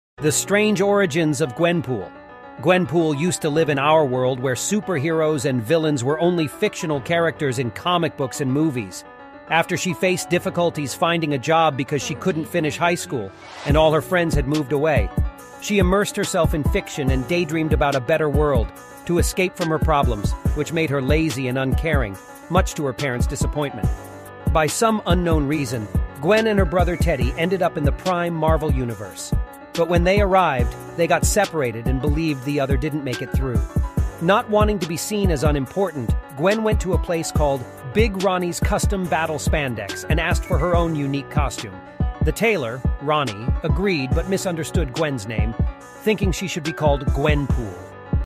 The Origins Of Gwenpool
Gwen Poole used to live in our world where superheroes and villains were only fictional characters in comic books and movies. After She faced difficulties finding a job because she couldn't finish high school, and all her friends had moved away. she immersed herself in fiction and daydreamed about a better world, To escape from her problems, which made her lazy and uncaring, much to her parents' disappointment. By some unknown reason, Gwen and her brother Teddy ended up in the prime Marvel Universe. But when they arrived, they got separated and believed the other didn't make it through. Not wanting to be seen as unimportant, Gwen went to a place called Big Ronnie's Custom Battle Spandex and asked for her own unique costume. The tailor, Ronnie, agreed but misunderstood Gwen's name, thinking she should be called "Gwenpool."

Background Music Credits To - OG PANDA BEATS